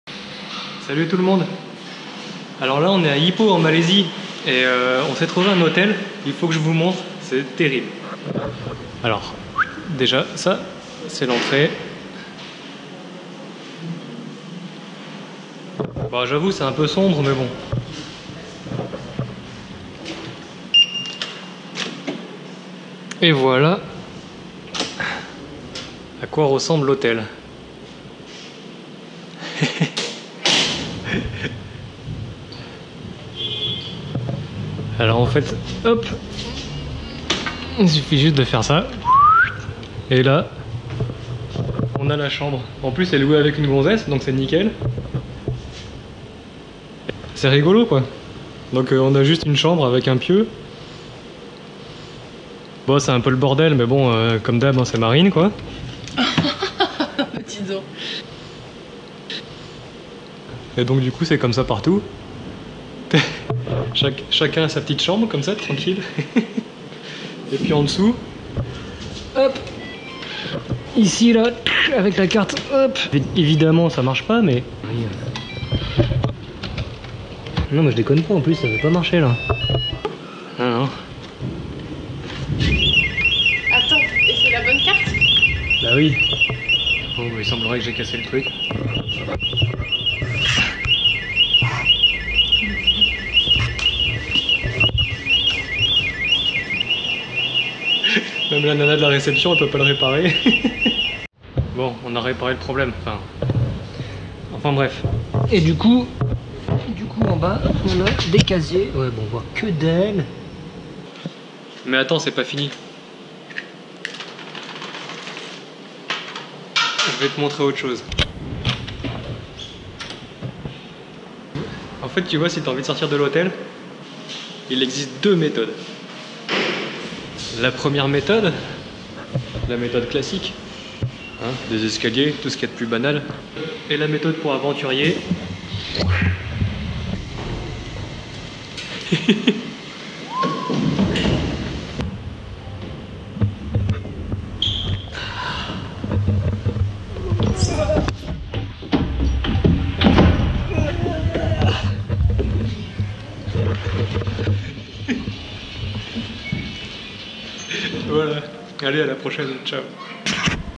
Hotel Container - Ipoh, Malaisie
Bonne découverte à Ipoh en Malaisie!! un hôtel container, notre premier!!

A bientôt... sur la route, on espère!

Flickr et 500 px : Rodolphe Bonneau